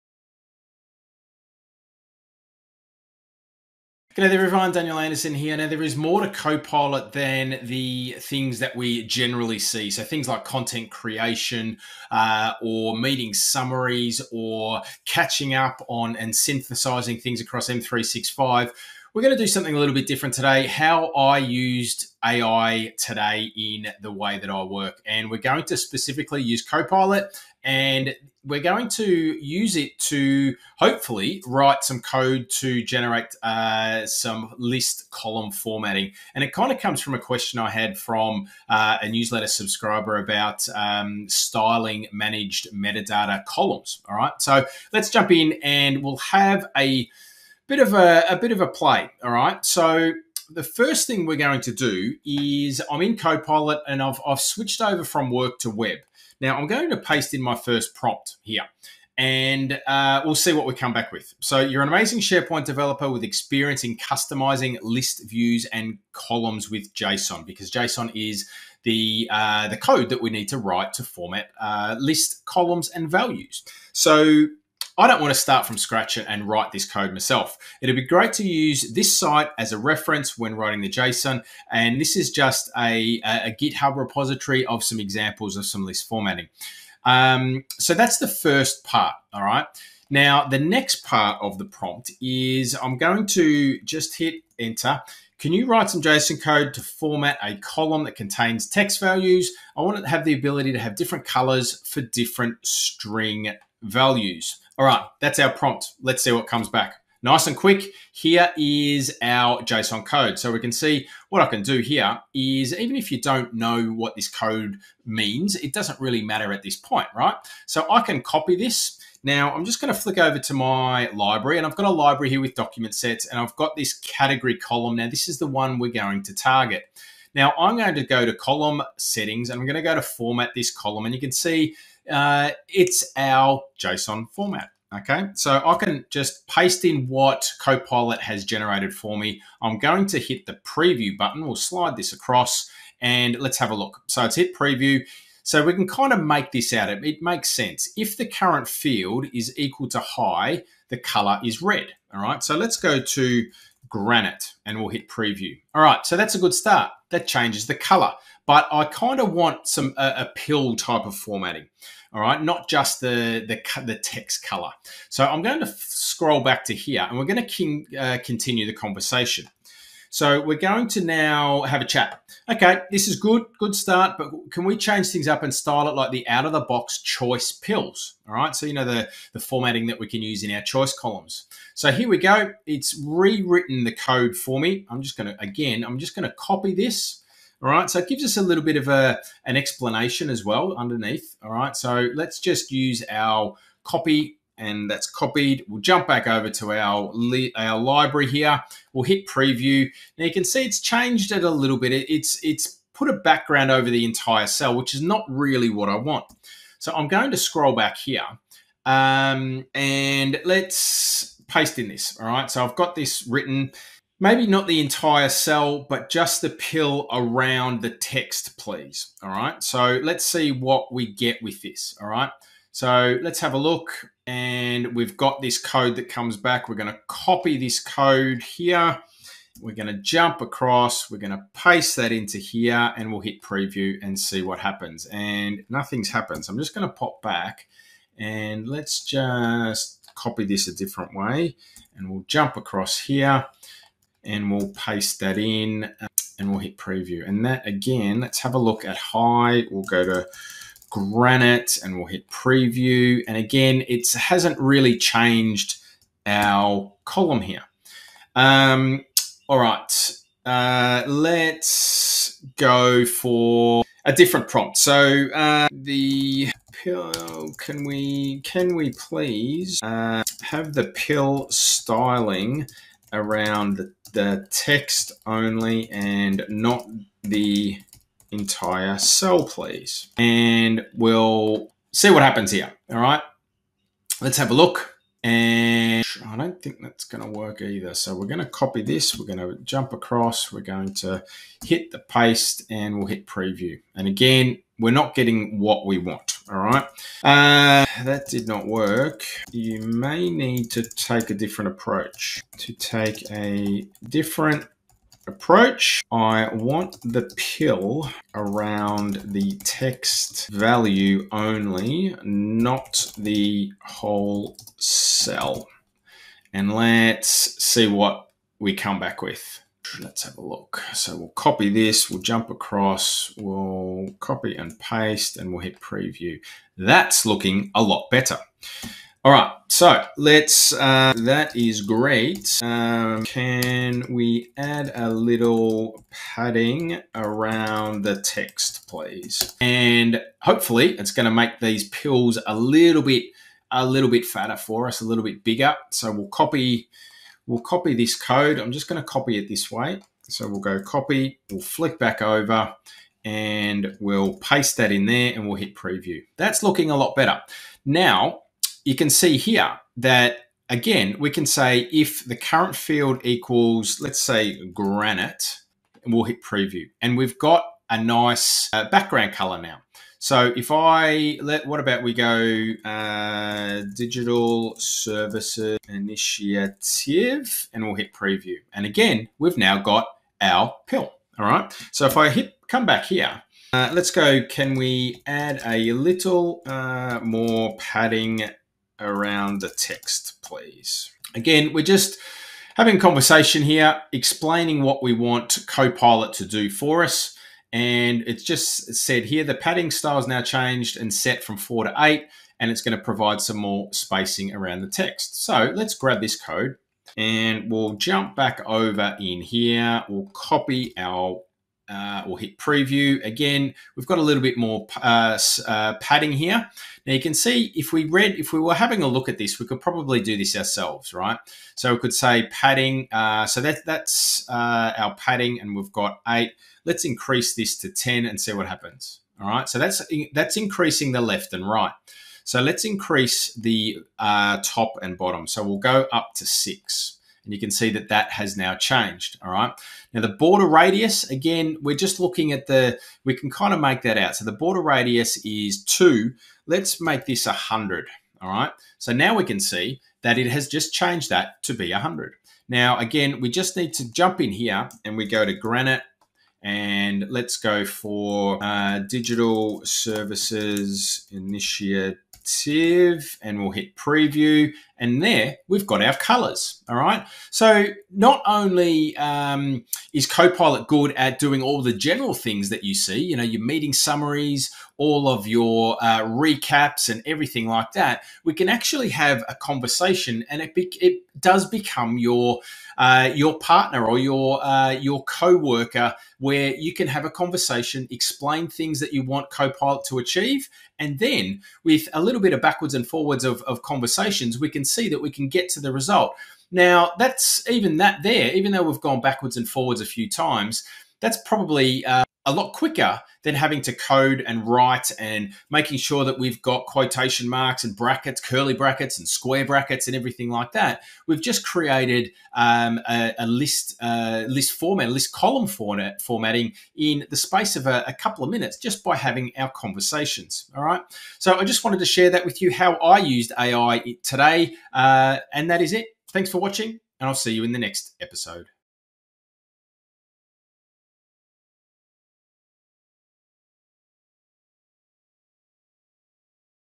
Stop Writing SharePoint JSON By Hand! (Copilot's Hidden Superpower) 🚀
Tired of writing complex JSON code for SharePoint column formatting?

Watch as I reveal how Microsoft Copilot can be your personal SharePoint developer! In this game-changing tutorial, I'll show you how to transform boring metadata columns into beautiful, colorful pills using simple conversations with AI.

🎯 What You'll Learn:

- How to leverage Copilot for SharePoint development
- Step-by-step JSON column formatting
- Creating beautiful pill-style metadata tags
- Advanced styling techniques without coding
- Pro tips for working with Copilot

⏱️ TIMESTAMPS:
00:00 - Introduction
00:52 - Setting up Copilot for SharePoint
01:45 - Writing our first JSON format
02:35 - Customizing column colors
03:27 - Creating pill-style formatting
05:15 - Fine-tuning the design
07:12 - Advanced styling tips
07:45 - Final results and demo

💡 PRO TIP: Save this video for reference - you'll want to come back to these techniques again and again!

Like it? every week this value is delivered via my emails. get them now and for free